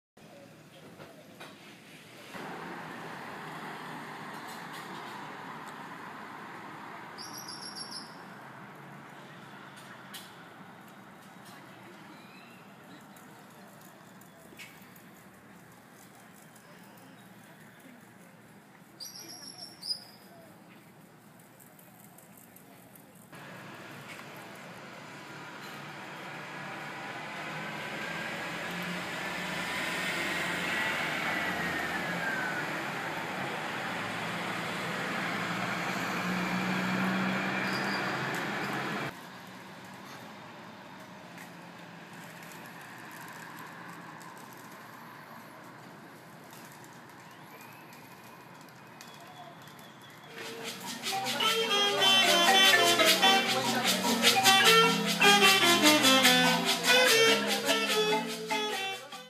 Easter in Merida, Yucatan, Mexico
I looked out our hotel window this morning to see that some of the streets were blocked off and pedestrians jogging, walking or riding bicycles.  I think there may be an Easter parade or procession planned.